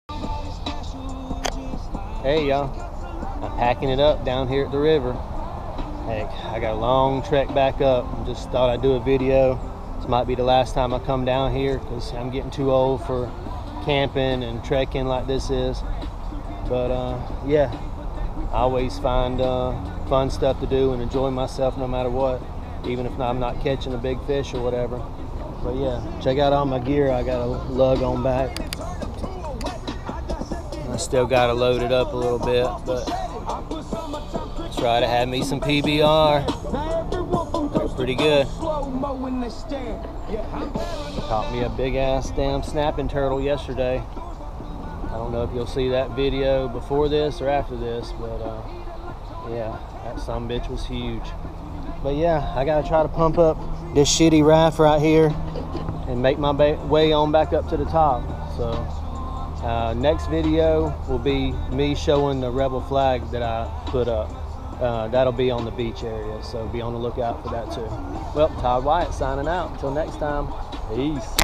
Last video before I pack it in
My last days down in the jungle. Live and raw 6/2021